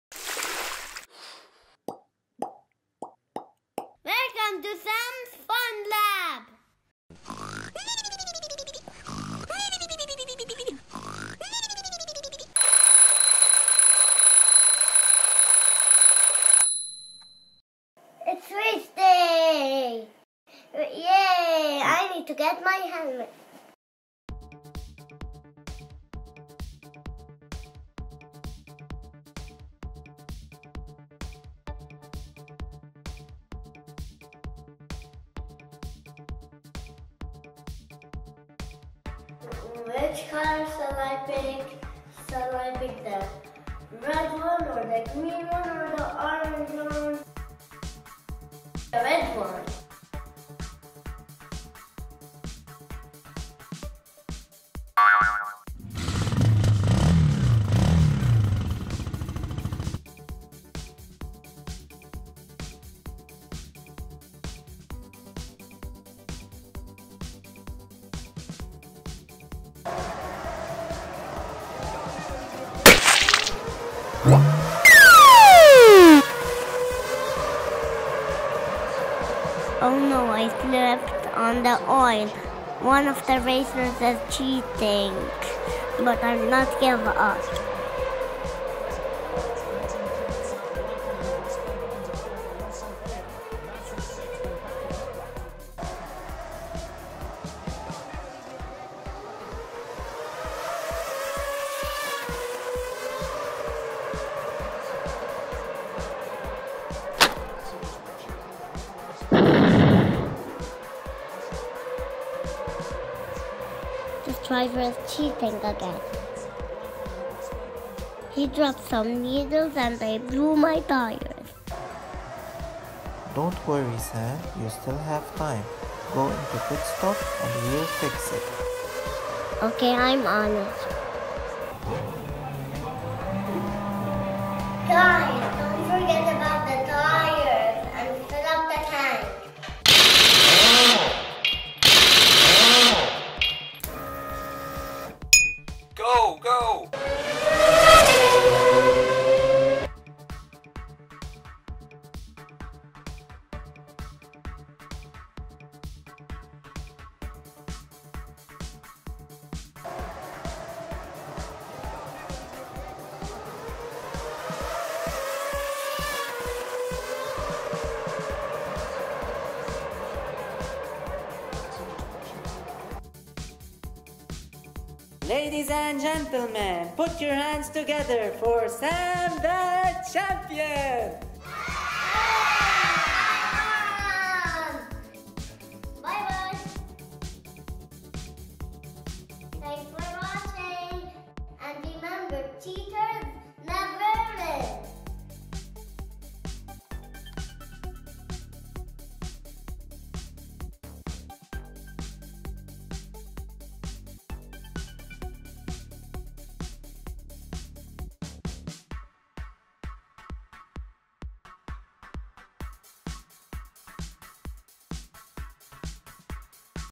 KIDS COMICS 🏁 Race Day
Sam i so excited about race day, he takes his helmet and heads over to pick his racing car. In the race, one driver i s cheating and he tries to put some obstacles to stop Sam from winning. Sam doesn't give up and eventually wins the race in this fun and fast episode.